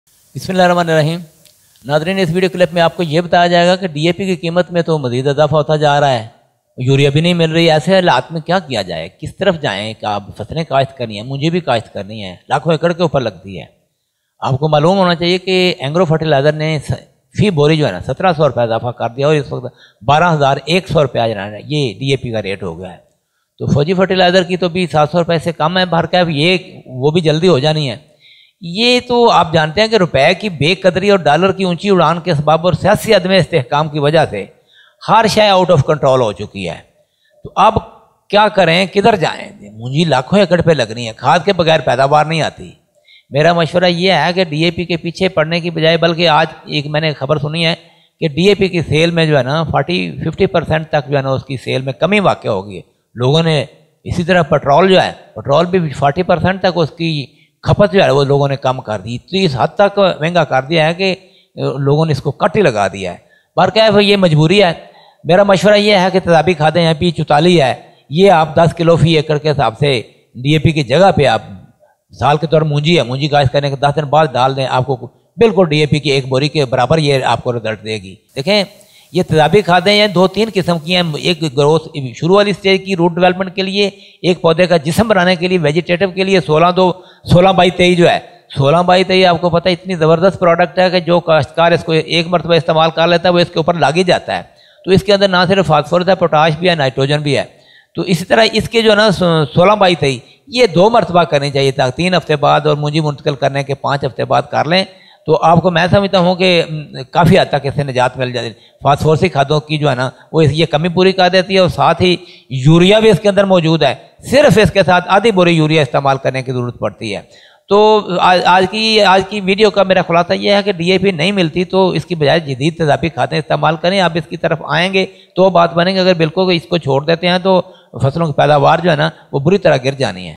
DAP price hike || Price of DAP has been increased || Crop Reformer
Recent price hike of DAP has made it impossible for small farmers to use even a single bag of dap fertilizer in their fields. New price of DAP is now 12100 per 50kg bag. In this video, we have tried to convey alternate of dap to be used in coming crops like rice.

These are some of my other relevant videos,

DAP (diammonium phosphate) is the king of artificial fertilizers and has been playing a very important role in the crop production worldwide. In Pakistan too, it is considered as the most important fertilizer for attaining the the potential yield and production of field crops. Its price has so much increases that it has become very difficult for the poor farmers to purchase this fertilizer. In this video, we have suggested the solophos (Jaffar AG), Ultrasol (Swat Agro) and Growfos as alternates of DAP. As these fertilizers are acidic in nature, readily soluble/available to the plant, therefore it is advised that these should be applied in split fertigation in the standing crop instead application as basal dose. Being acidic in nature and more rich in phosphorus, MAP (mono ammonium phosphate) of Engro has also been declared as better option than DAP.